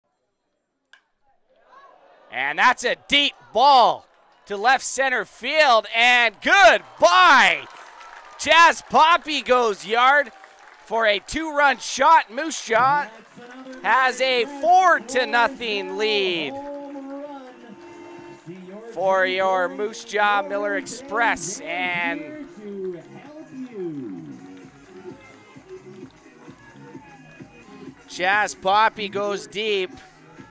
Chaz Poppy Home Run July 5th 2023
#33 Chaz Poppy Home Run vs the Okotoks Dawgs  July 5th, 2023
Production Red Dog Visual